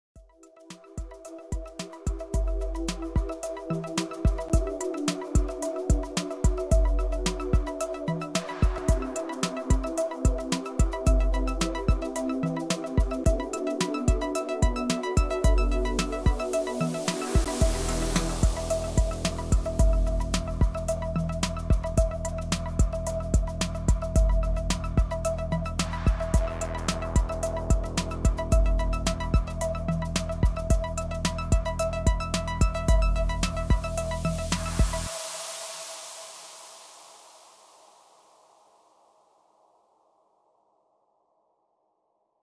SIA "Grobinas Meliorators"
Advertisement video for Latvian agriculture company.